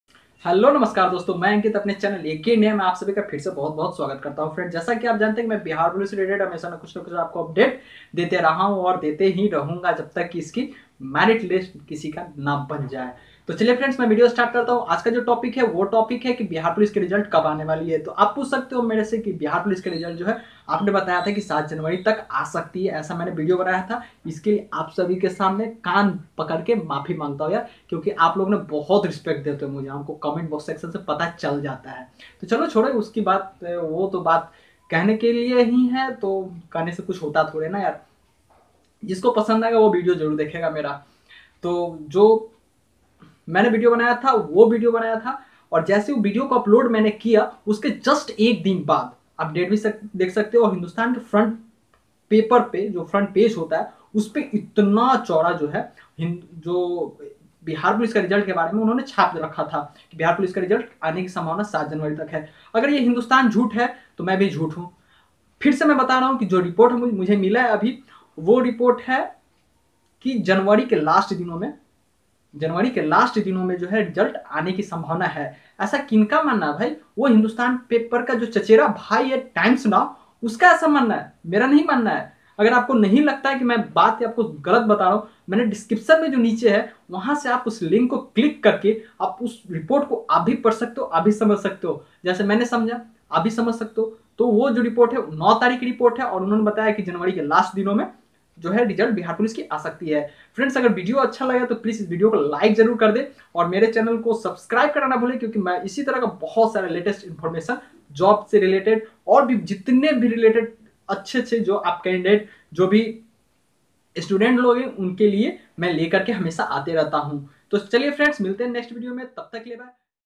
Bihar Police Result | Bihar Police Result Date | बिहार पुलिस रिजल्ट 2018 | Ak india,TimesNow
बिहार पुलिस रिजल्ट 2018!
Bihar Police result date maine iss video mai btaya hi. Bihar police result ki sambhawit date ka jikr kiya hu. Aisa news paper TimesNow ka btana hai mera nii.
Niche jo link diya huaa hai waha click krke aap dekh skte hii wo report ko.

agr video pasand aaya to plz like kr dijiye, or apne friend circle mai share krna na bhule.